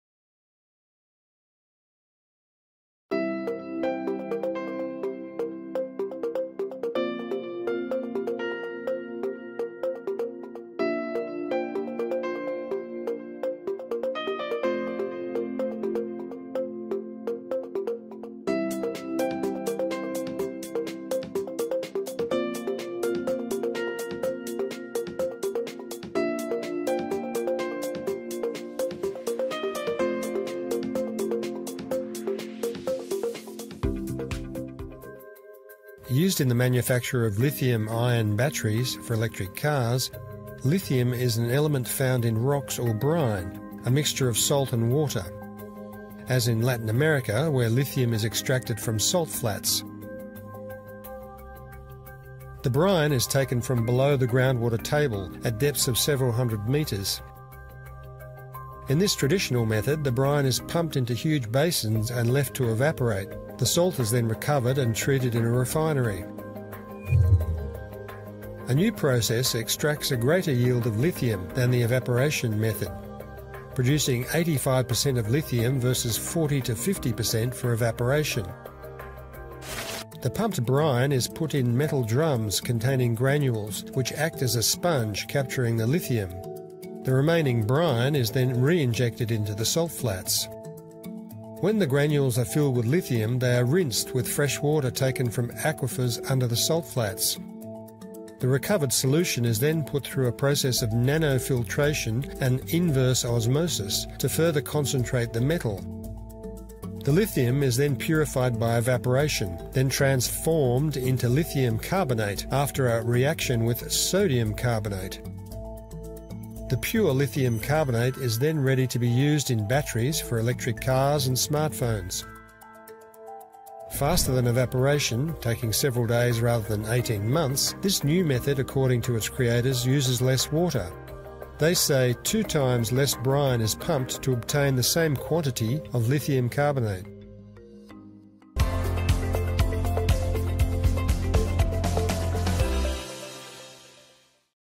นวัตกรรม ‘วิธีสกัดลิเธียมโดยตรง’ ทางแก้ลิเธียมขาดแคลน
นักวิจัยเพิ่งออกมาเตือนว่า โลกกำลังจะขาดแคลน ‘ลิเธียม’ ใน 2 ปี หลังความต้องการรถยนต์ไฟฟ้าพุ่งไม่หยุด แต่เมื่อเร็วๆนี้ นักวิทยาศาตร์ก็ได้ค้นพบ นวัตกรรมใหม่ ที่จะช่วยให้สกัดลิเธียมได้มากขึ้น และเร็วขึ้นแล้ว